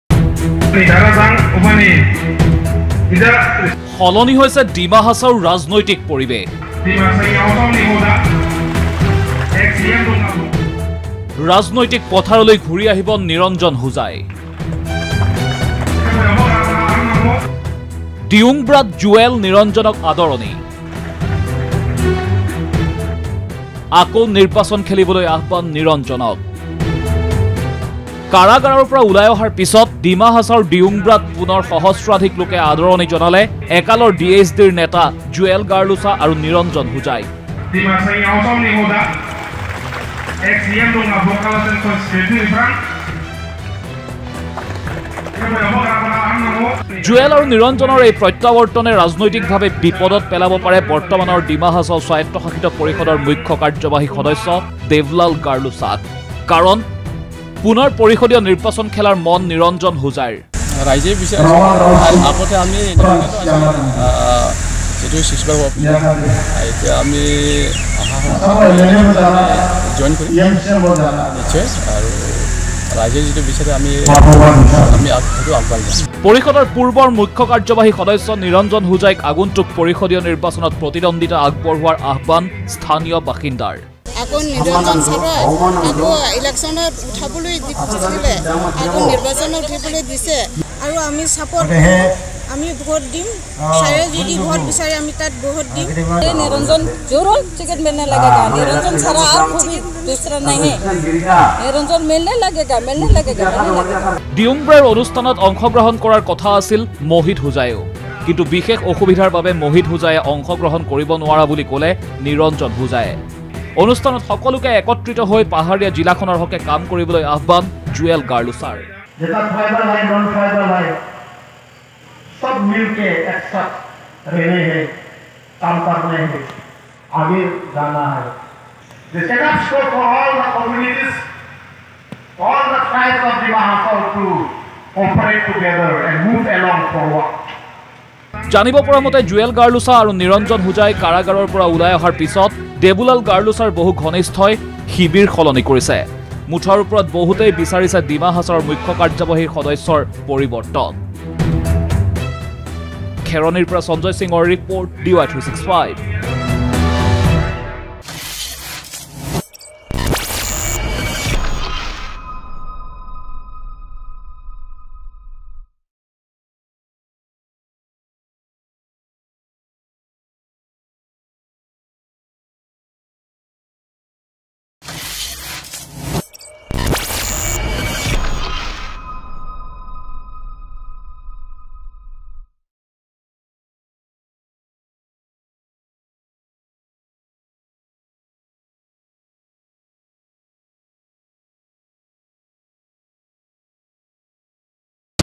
সলনি হৈছে ডিমা হাছাওৰ ৰাজনৈতিক পৰিৱেশ
ডিমা হাছাওৰ দিয়ুংব্ৰাত সহস্ৰাধিক লোকে আদৰণি জনালে এসময়ৰ DHD (J)ৰ নেতা জুৱেল গাৰ্লোচা, নিৰঞ্জন হোজাইক। আদৰণি অনুষ্ঠানত পুনৰ পৰিষদীয় নিৰ্বাচনত অৱতীৰ্ণ হোৱাৰ ইচ্ছা ব্যক্ত কৰিলে ডিমা হাছাও স্বায়ত্ত শাসিত পৰিষদৰ পূৰ্বৰ মুখ্য কাৰ্যবাহী সদস্য নিৰঞ্জন হোজায়ে।